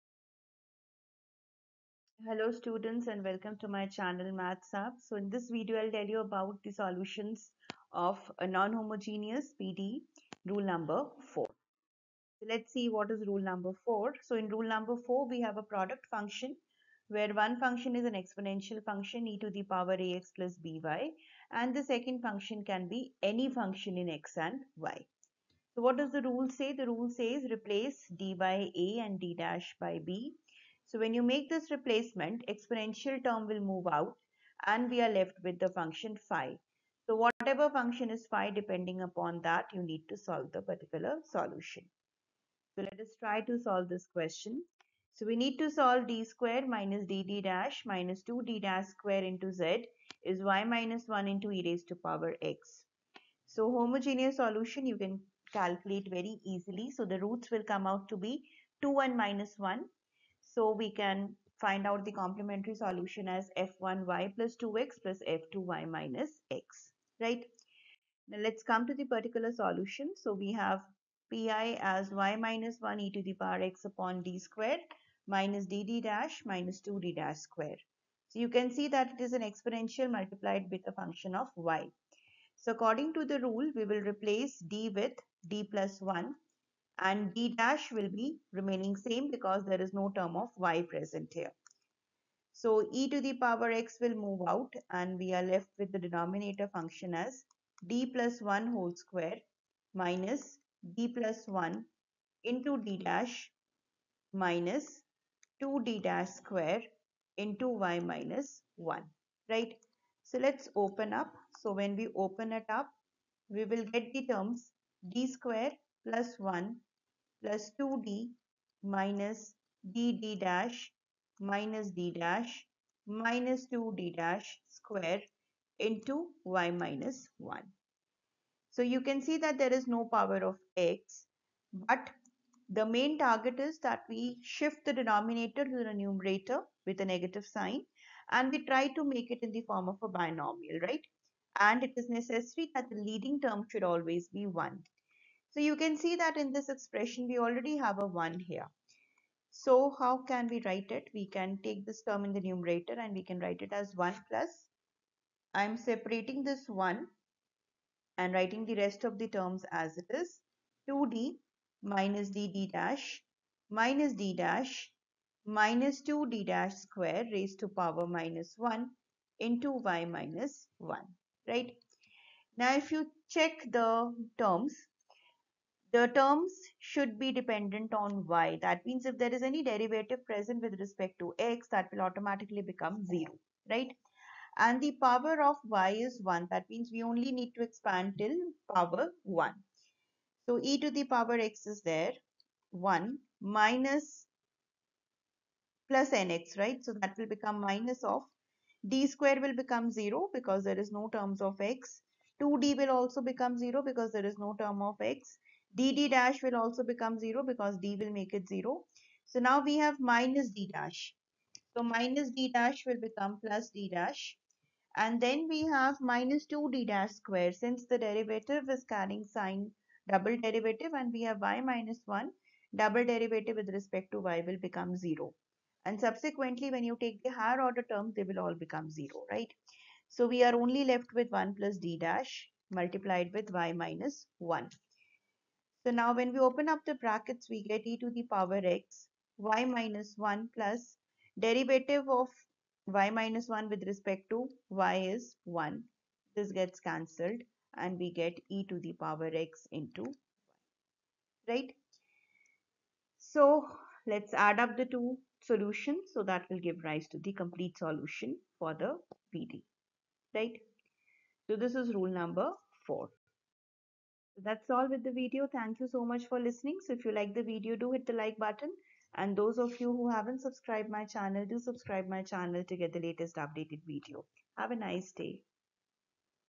Particular Solution-Rule 4-Part-9 (Partial Differential Equations)
This video series will focus on Second Order PDE with constant coefficients-Particular Solution Rule4. Examples are solved to explain each and every concept.